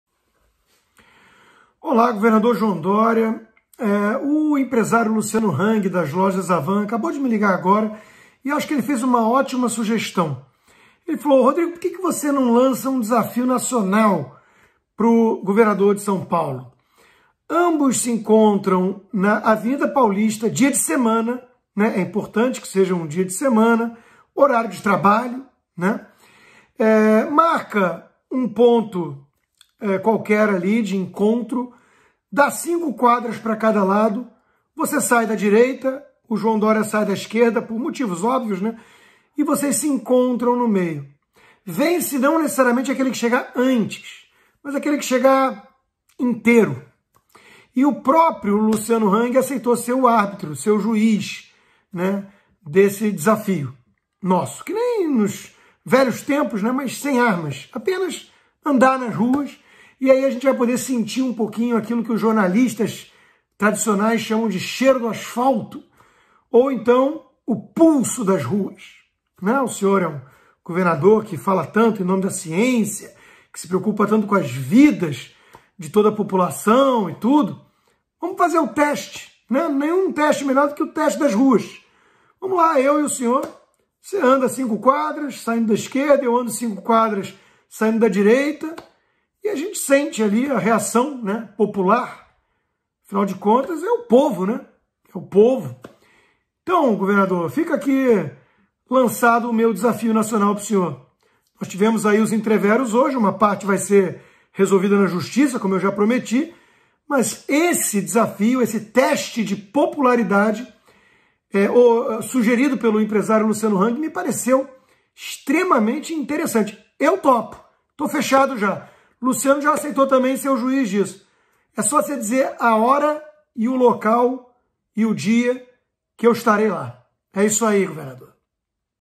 RODRIGO CONSTANTINO DESAFIA JOÃO DORIA PARA UM TESTE DE POPULARIDADE. QUEM CHEGAR INTEIRO VENCE!
Rodrigo Contatino desafia João Doria para um teste de popularidade. Quem chegar inteiro vence!